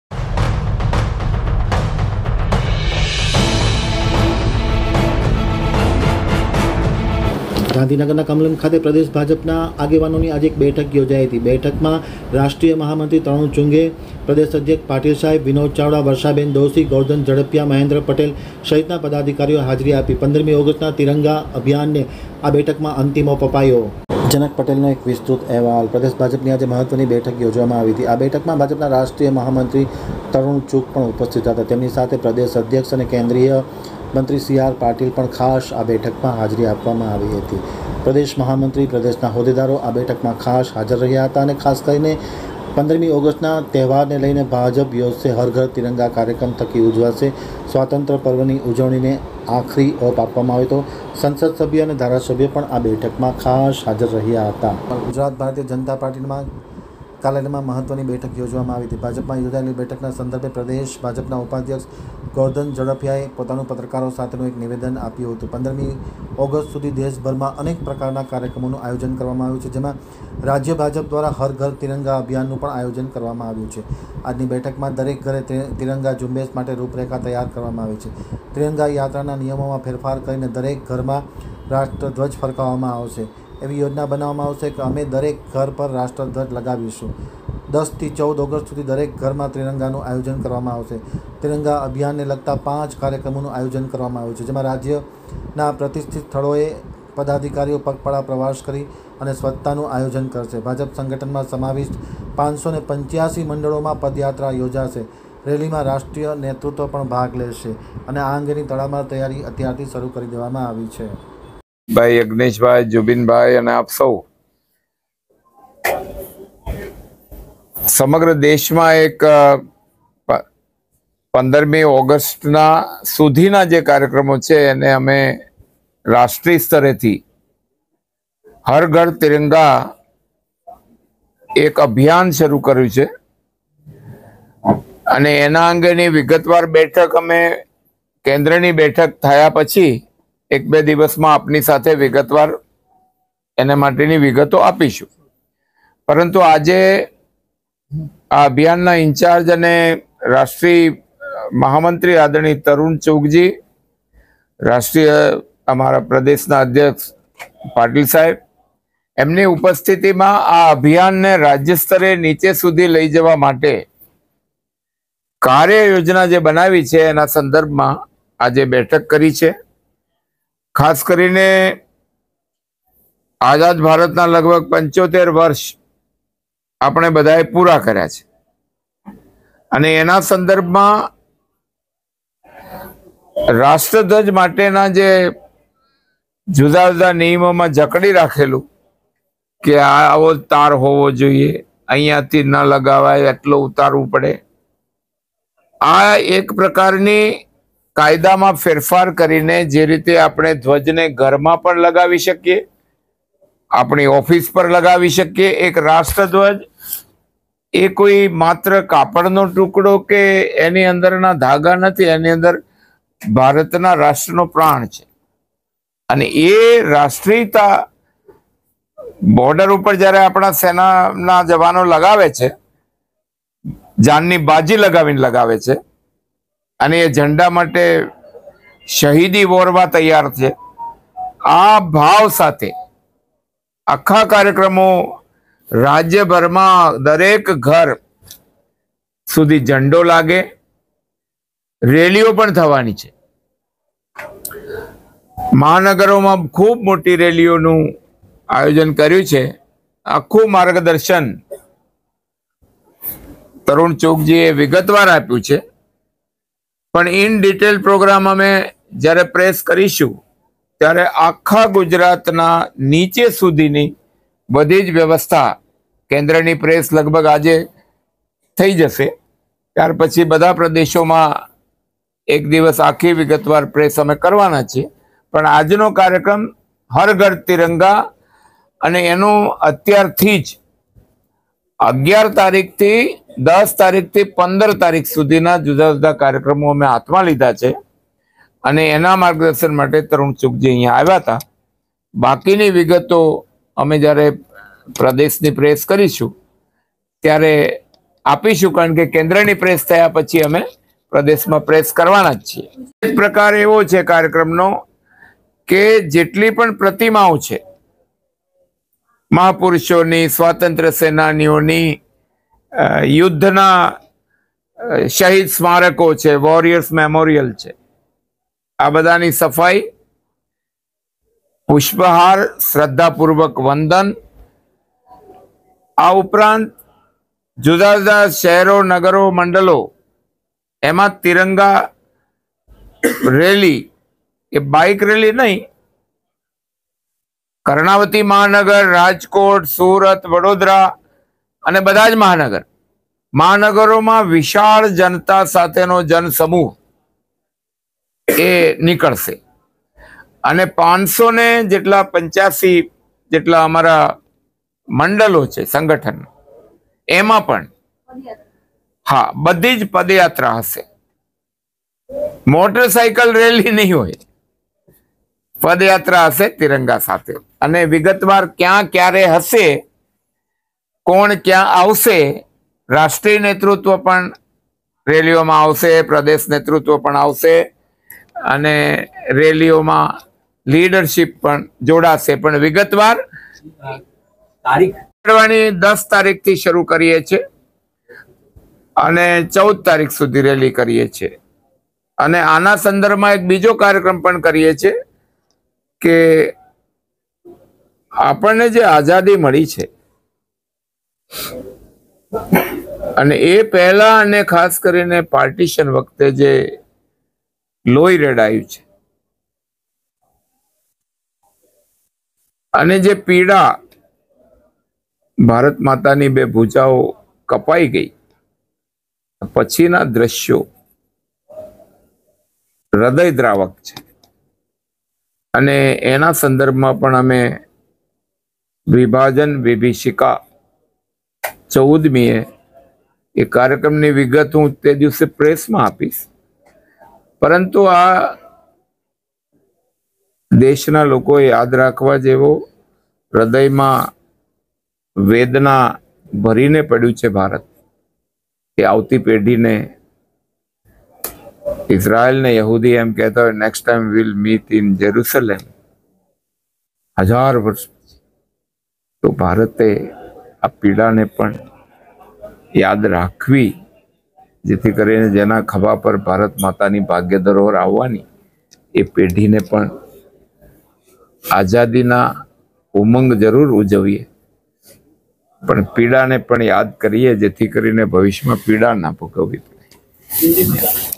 AAMTAK_NEWS કચ્છ સહિત રાજ્યમાં તારીખ 10 થી 14 સુધી હર ઘર તિરંગા અભિયાન અને 585 મંડળમાં પદયાત્રા
ગાંધીનગર

 ગુજરાત ભારતીય જનતા પાર્ટી કાર્યાલયમાં મહત્વની બેઠક યોજાઈ

 ભાજપમાં યોજાયેલી બેઠક સંદર્ભે પ્રદેશ ભાજપ ઉપપ્રમુખ ગોરધન ઝડફિયાનું નિવેદન

 15 ઓગસ્ટ સુધી દેશભરમાં કાર્યક્રમો યોજાશે

 રાજ્ય ભાજપ દ્વારા હર ઘર તિરંગા અભિયાનનું આયોજન કરવામાં આવ્યું છે.

 આજની બેઠકમાં દરેક ઘરે ત્રિરંગા ઝુંબેશ માટે રૂપરેખા તૈયાર કરવામાં આવી હતી.

  તિરંગા યાત્રાના નિયમોમાં ફેરફાર કરીને દરેક ઘરમાં રાષ્ટ્રધ્વજ ફરકાવી શકાશે!

 એવી યોજના બનાવવામાં આવશે કે અમે દરેક ઘર પર રાષ્ટ્રધ્વજ લગાવીશું

 10 થી 14 ઓગસ્ટ સુધી દરેક ઘરમાં ત્રિરંગાનું આયોજન કરવામાં આવશે

 તિરંગા અભિયાનને લગતા પાંચ કાર્યક્રમો

 રાજ્યના પ્રતિષ્ઠિત સ્થળોએ પગપાળા પ્રવાસ કરીને પણ સ્વચ્છતા કરવામાં આવશે.

 ભાજપ સંગઠનમાં સમાવિષ્ટ 585 મંડળોમાં પદયાત્રા યોજાશે

 રેલીમાં રાષ્ટ્રીય નેતૃત્વ પણ ભાગ લેશે

 બાઈટ-: ગોરધન ઝડફિયા- ઉપપ્રમુખ ભાજપ